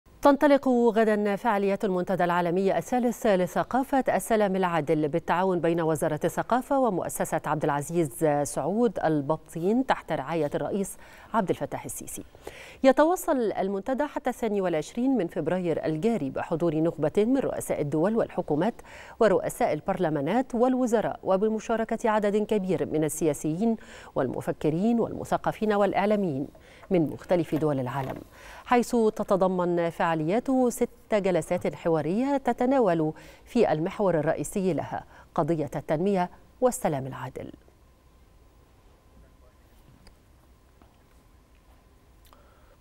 غدا.. انطلاق فعاليات "المنتدى العالمي الثالث لثقافة السلام العادل" تحت رعاية الرئيس السيسي
نشرة الثالثة | غدا.. انطلاق فعاليات "المنتدى العالمي الثالث لثقافة السلام العادل" تحت رعاية الرئيس السيسي